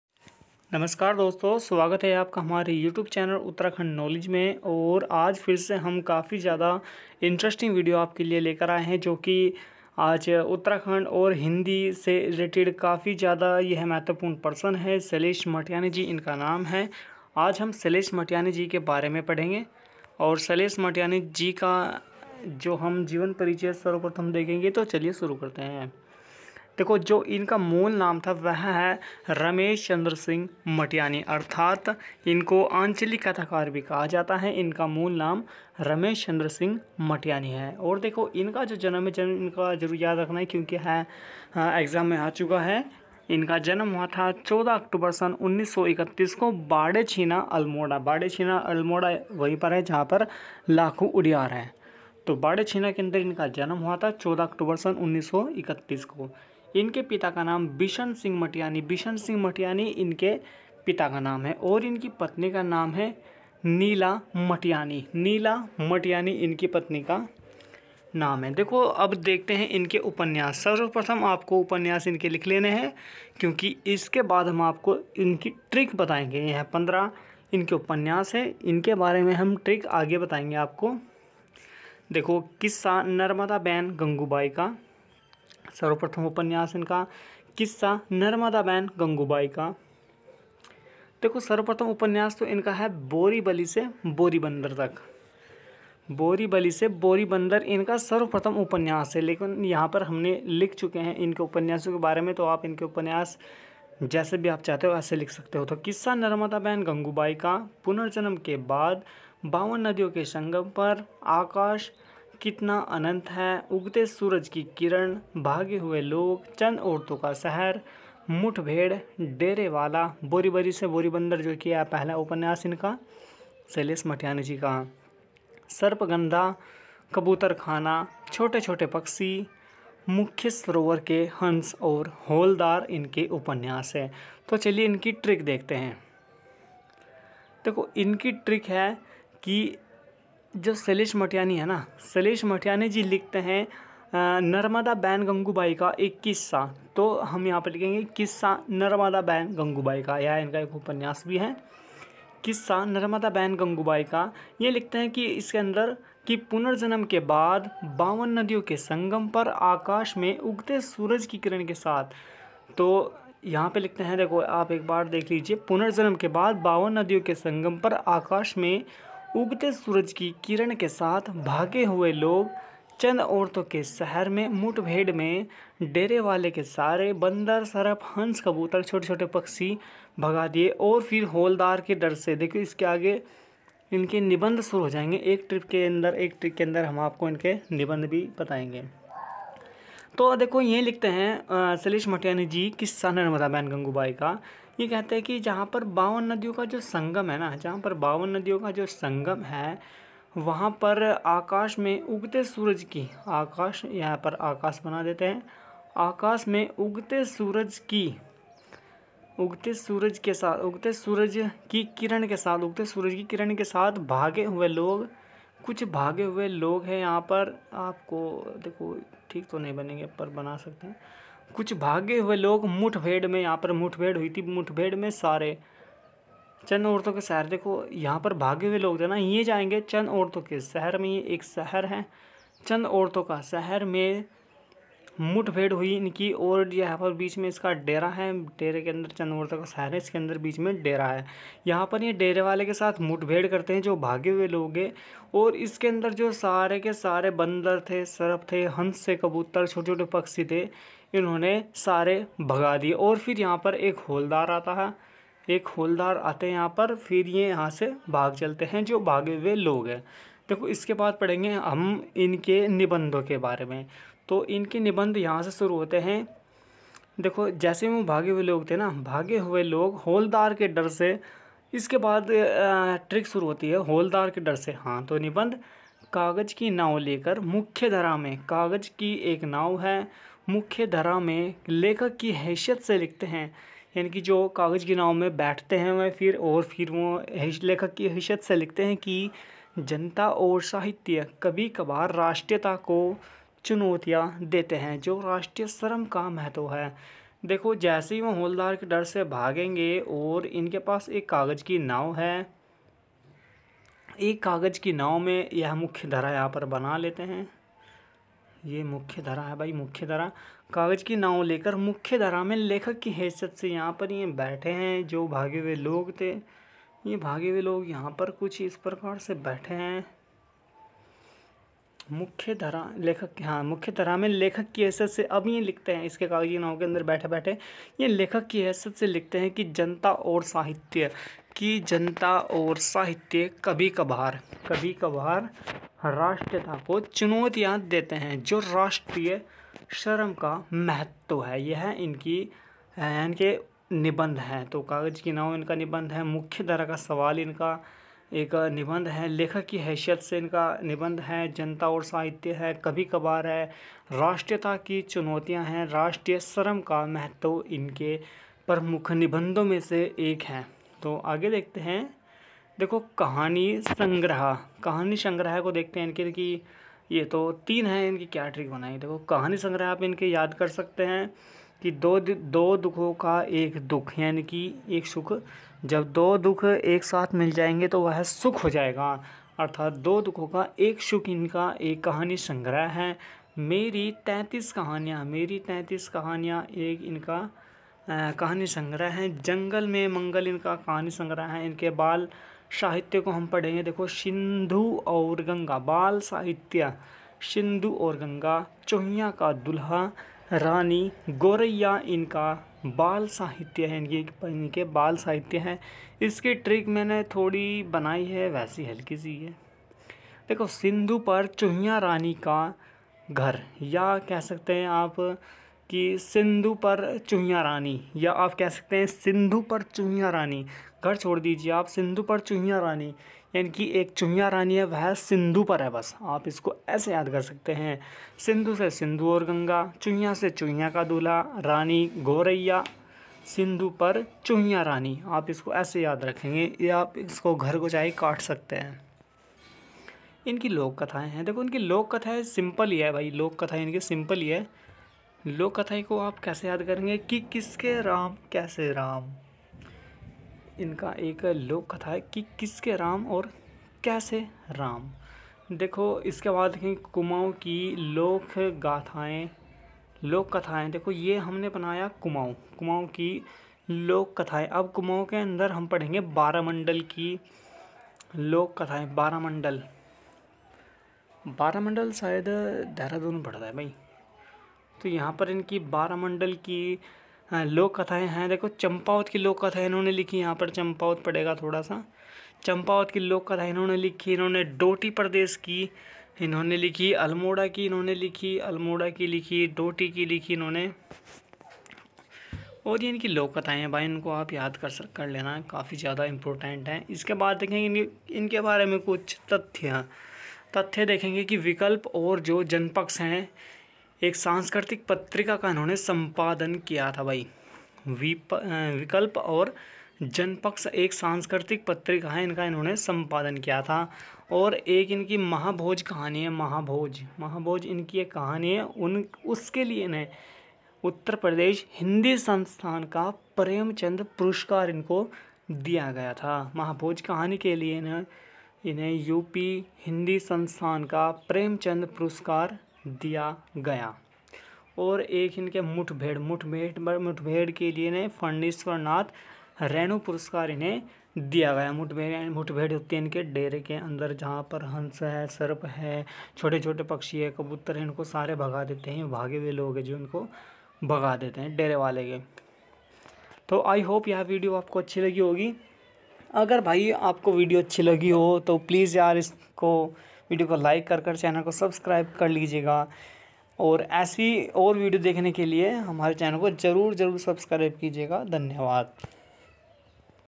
Shailesh matiyani ji ki rachnaen with tricks
Shailesh matiyani ji ki rachnaen vah bhi trick ke sath.
Jivan Parichay
Upnyas
Nibandh
Kahani sangrah
Bal sahitya
Kathaen
Vishesh tathya